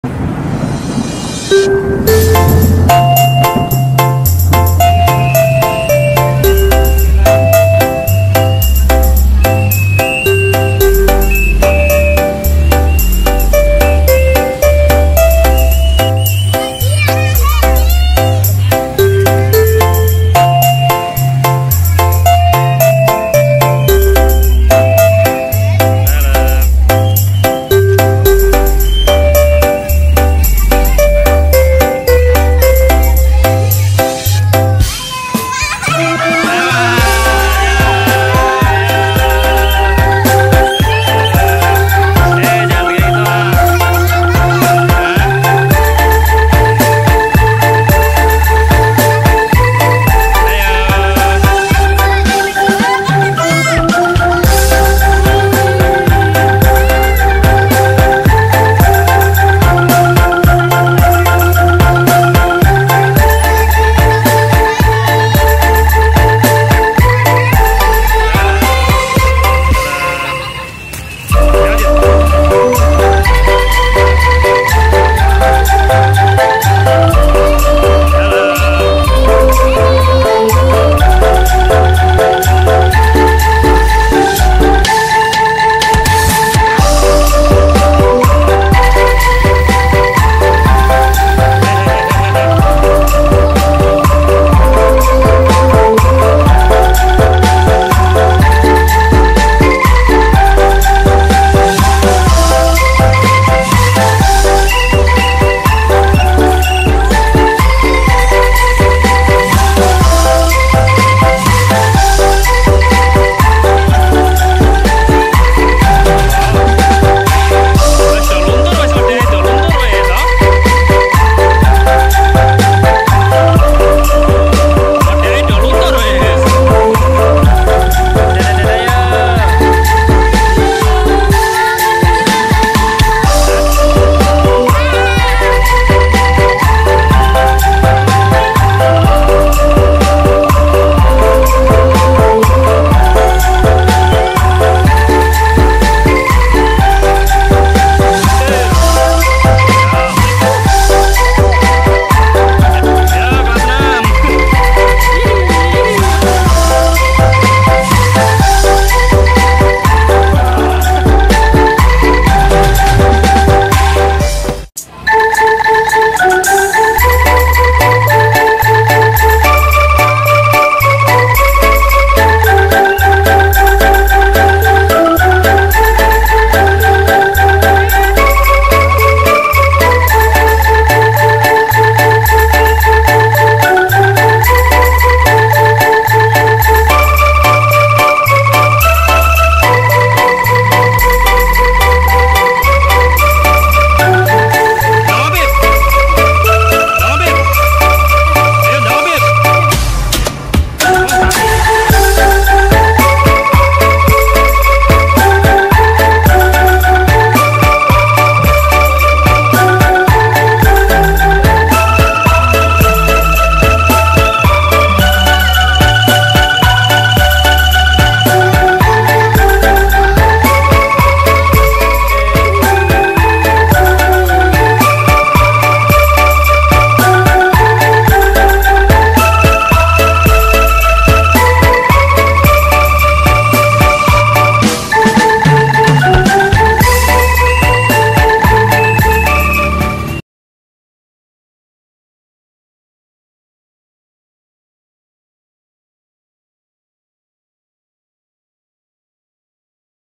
KARNAVAL HUT Kemerdekaan RI SDN KURIPAN LOR 01
Karnaval Budaya SDN Kuripan Lor 01 dalam rangka memperingati HUT RI ke-79 tanggal 20 Agustus 2024
Semua siswa SDN Kuripan Lor 01 yang berjumlah 191 anak, mulai kelas 1-6 mengenakan baju kreasi sendiri maupun baju adat dari berbagai daerah.
simak keseruannya.
PART 1